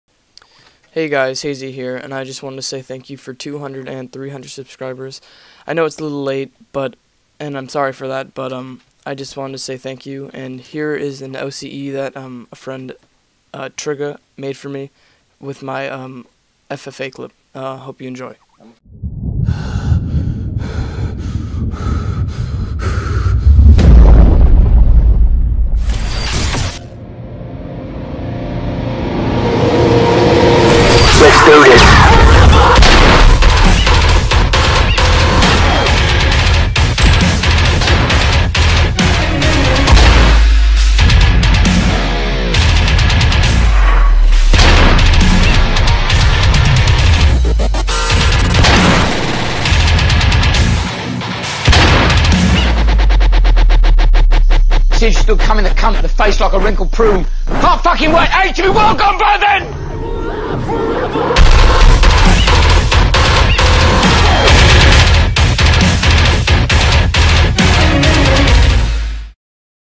Focus Hazy | Thanks for 200/300 subs!!!
I know it's a little late and all, but i wanted to say thank you for all the support ive been receiving over the last couple weeks. TRIGGA made an amazing OCE for me, so i hope you enjoy that too.